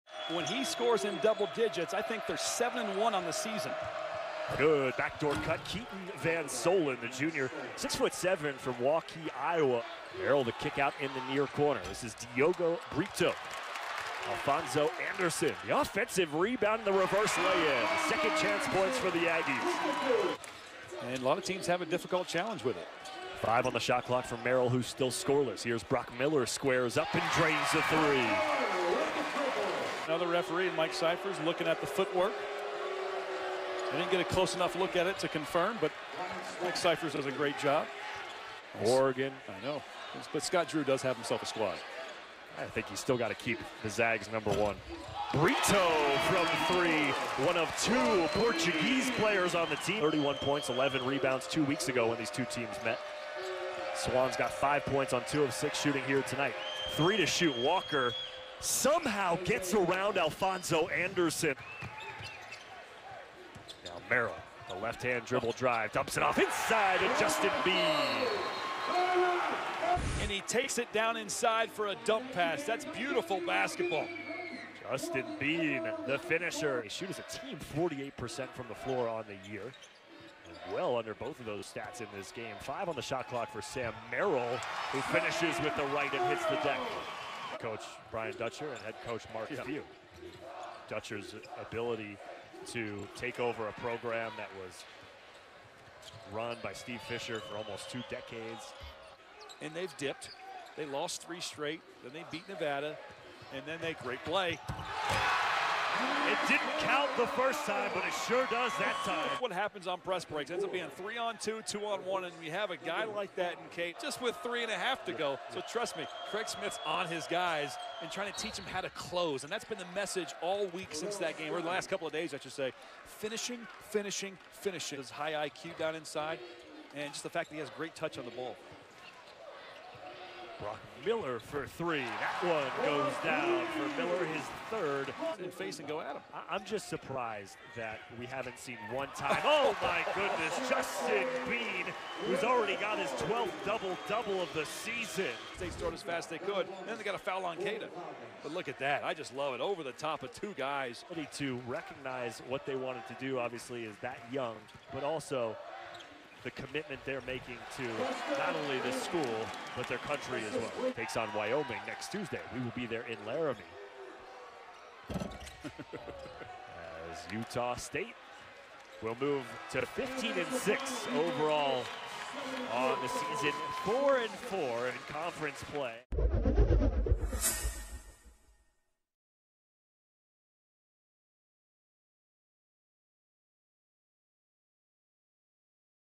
Air Force vs. Utah State Basketball Highlights (2019-20) | Stadium
Utah State finished the first half on a 12-4 run to build up an 11 point lead, and cruised in the second period as they blew passed Air Force 72-47. Check out the highlights from the Aggies' dominant win in Logan on Tuesday night here!